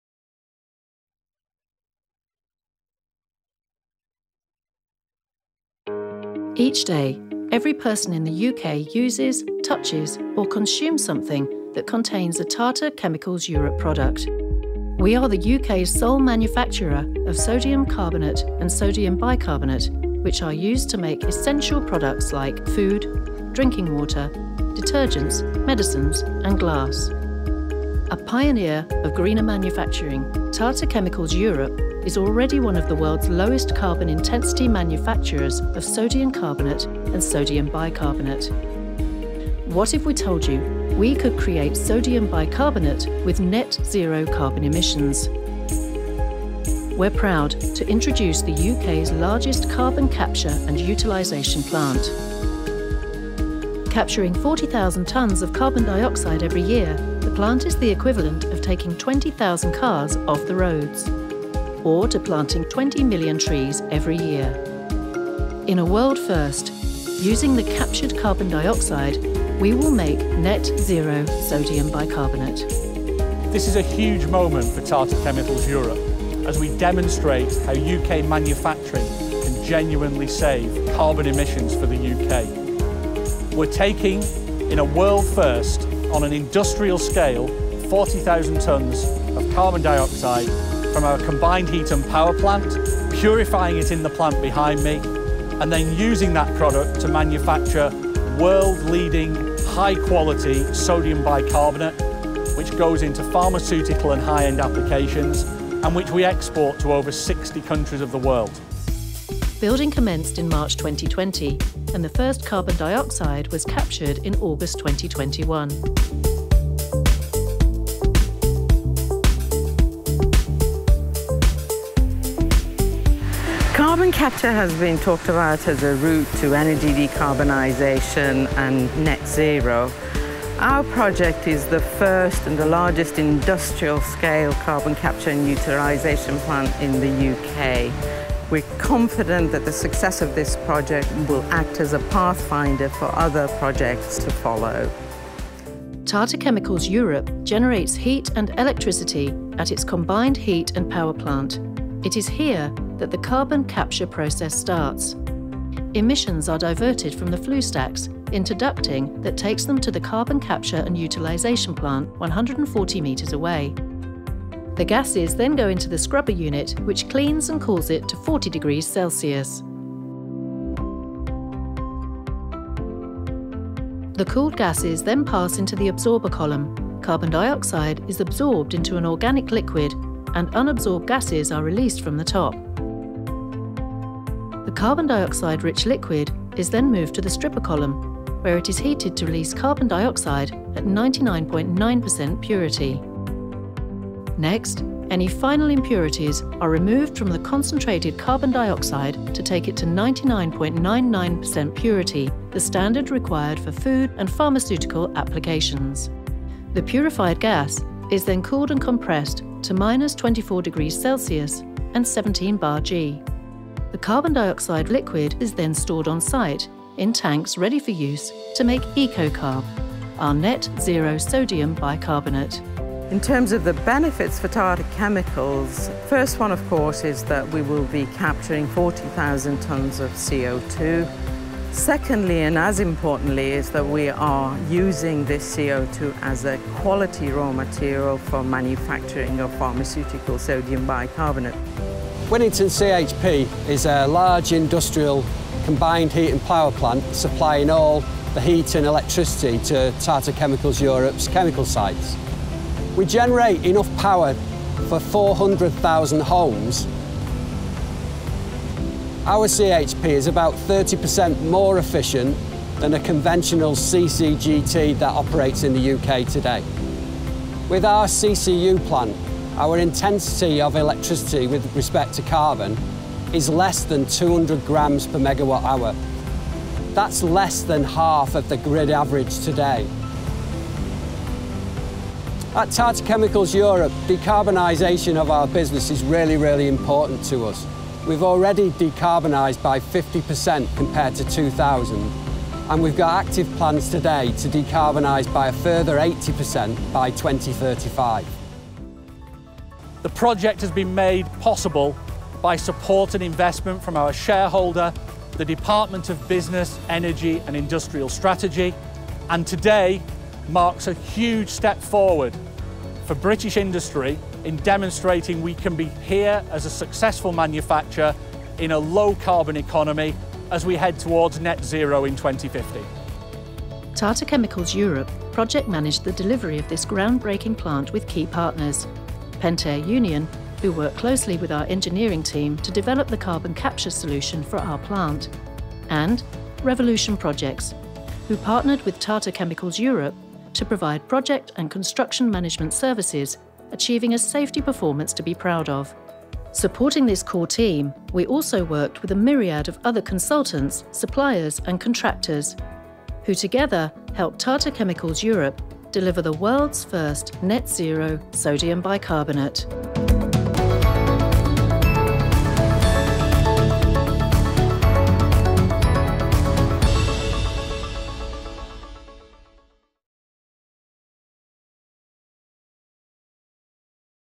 Tata Chemicals Europe Opens UK's largest industrial scale carbon capture and utilisation plant
Watch this video to find out more about the Tata Chemicals Europe carbon capture and utilisation plant.

Tata Chemicals Europe is now manufacturing one of the lowest carbon footprint sodium bicarbonate and sodium carbonate products in the world. These chemicals are used to make a wide array of everyday items seen in many households.

Already generating heat and electricity from one of the most efficient combined heat and power plants in the UK, the carbon footprint of our operations in Northwich has been significantly reduced following the completion of the UK’s only industrial scale carbon capture and utilisation (CCU) plant. This plant will enable more than 10% of carbon dioxide produced by our combined heat and power plant (CHP) to be recycled, purified and used as a key raw material to produce high grade sodium bicarbonate.

In June 2022, Mike Amesbury, MP for Weaver Vale, officially opened the UK’s first and largest industrial scale carbon capture and utilisation plant, enabling us to make the world’s first carbon neutral sodium bicarbonate. Supported by a £4.2m grant from the Department for Business, Energy and Industrial Strategy (BEIS), the £20m investment at our Winnington site captures 40,000 tonnes of carbon dioxide each year - the equivalent of taking 20,000 cars off the roads.

In a world first, carbon dioxide captured from energy generation emissions is then purified to food and pharmaceutical grade for use as a raw material in the manufacture of sodium bicarbonate, which will be known as EcoKarb® and will be exported to over 60 countries around the world. Much of the sodium bicarbonate exported will be used in haemodialysis to treat people living with kidney disease.

“The CCU demonstration plant enables us to reduce our carbon emissions, whilst securing supplies of a critical raw material, helping to grow the export of our products across the world."

Martin Ashcroft, Managing Director of Tata Chemicals Europe